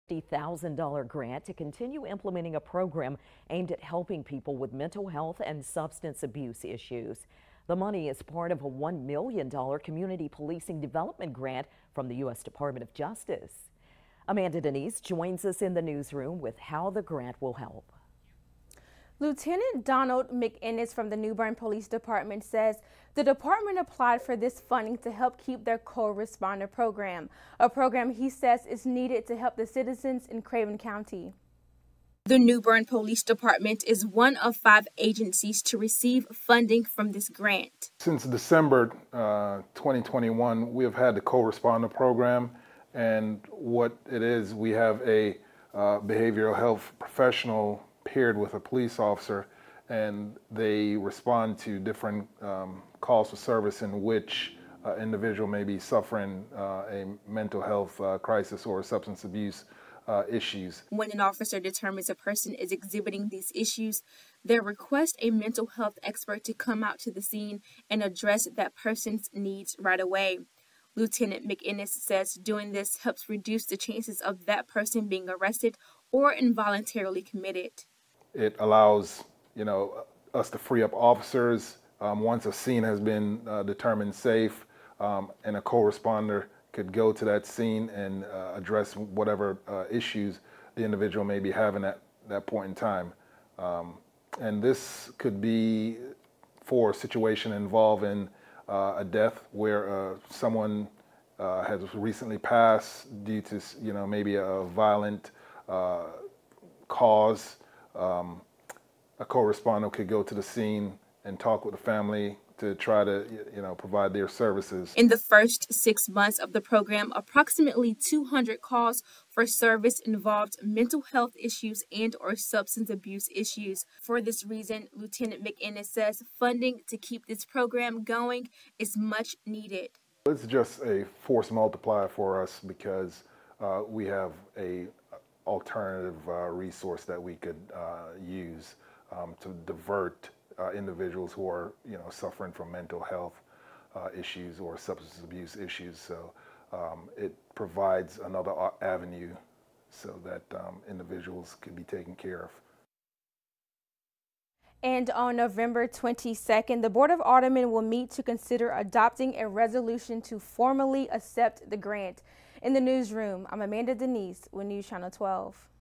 New Bern Police to receive grant from U.S. Department of Justice
The New Bern Police Department has been awarded a $350,000 grant to continue implementing a program aimed at helping people with mental health and substance abuse issues.